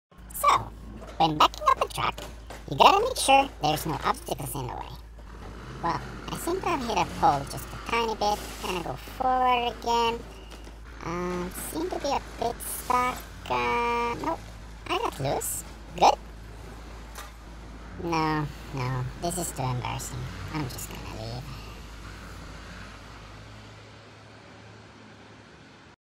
Car Crashes - How to back up a big truck
In this lesson, we'll be going through the necessary steps to backing up a big truck. There's a few things to be mindful of, but after watching this video, even your mother-in-law will be begging you to back up her truck!